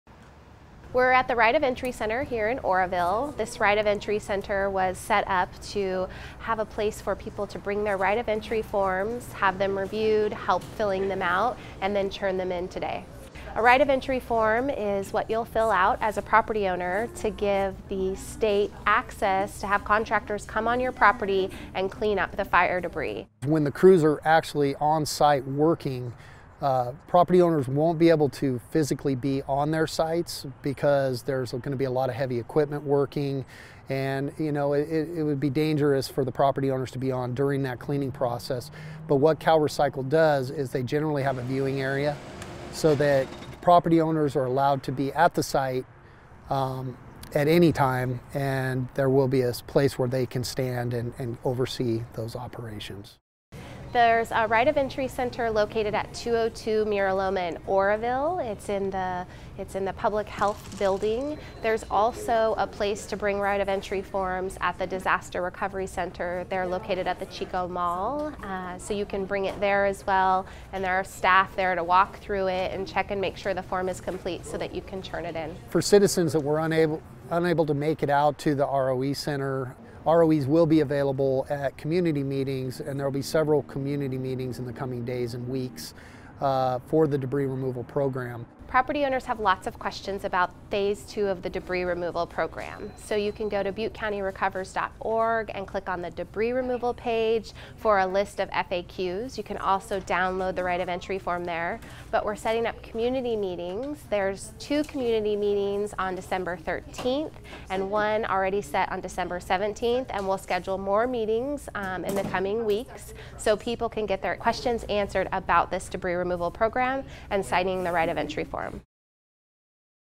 Right of Entry Centers Open in Oroville and Chico
Cal OES, in conjunction with Butte County and the Town of Paradise, are facilitating safe removal and handling of burn debris and ash related to the Camp Fire. Homeowners must complete the proper paperwork to allow agencies to clean up their properties. That process begins here at the Right-of-Entry center.